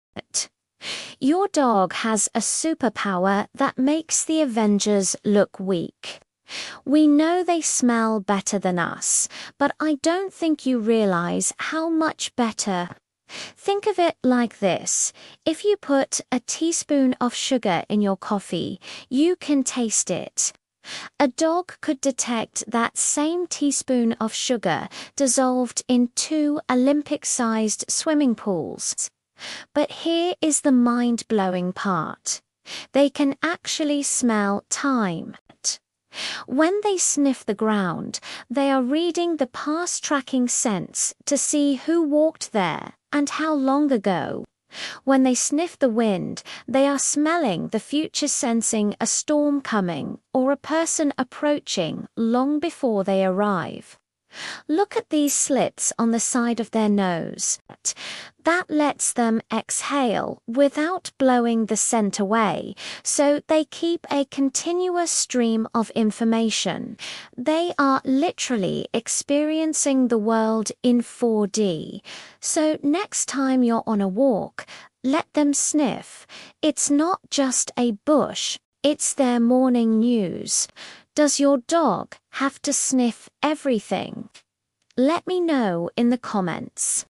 Your Dog Can Smell TIME
Did you know your dog is basically a time traveler?

We know dogs have good noses, but the science is actually insane. If you could taste a teaspoon of sugar in your coffee, a dog could taste that same teaspoon dissolved in TWO Olympic-sized swimming pools.

But the craziest part? They can smell time. The Past: Tracking heat on the ground to see who was there. The Future: Sniffing the wind to sense storms or people coming.

Next time they stop to sniff a bush, let them do it! They are just reading the morning news. 📰

Does your dog have to sniff EVERY blade of grass on a walk? Tell me below! 👇